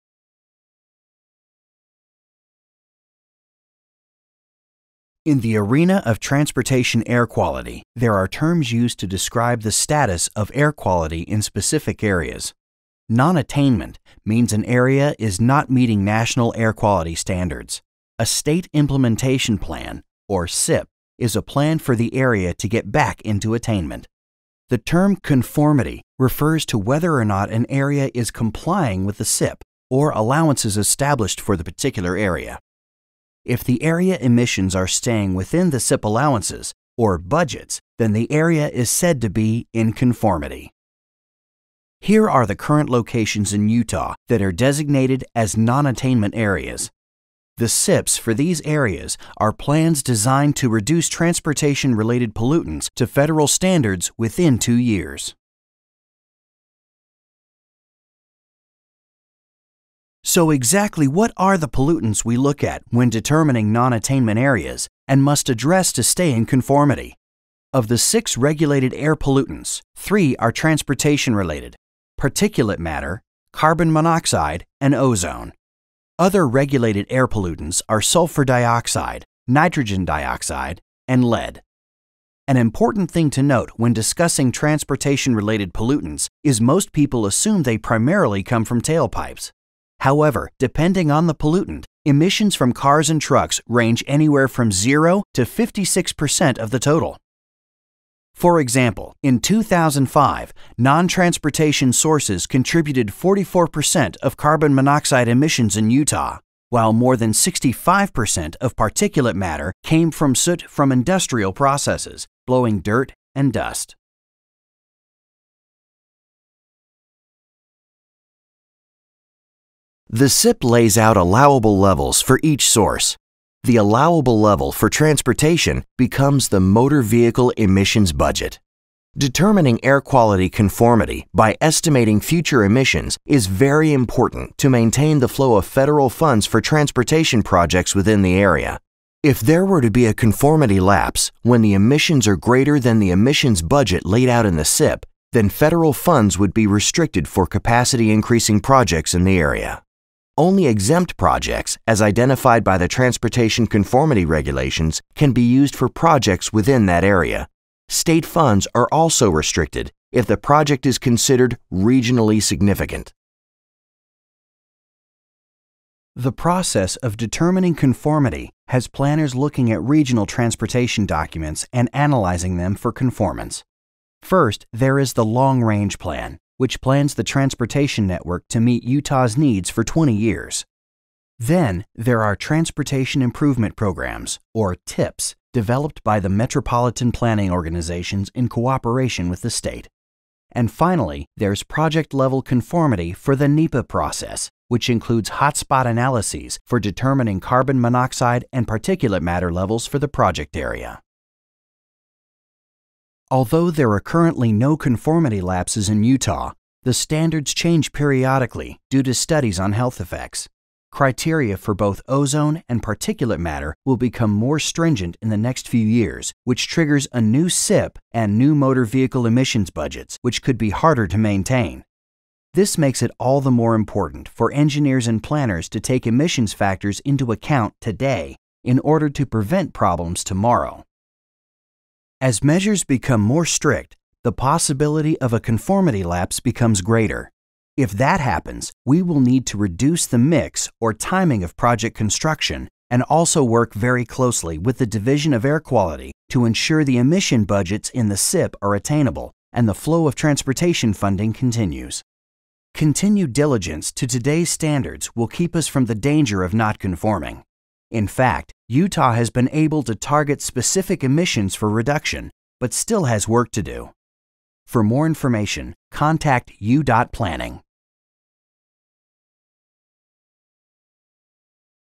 HD-Air Quality and the Transportation Conformity Process
Air Quality and the Transportation Conformity Process - high definition video explaining how Utah and other states must conform to the EPA's air quality standards in order to receive funding for transportation.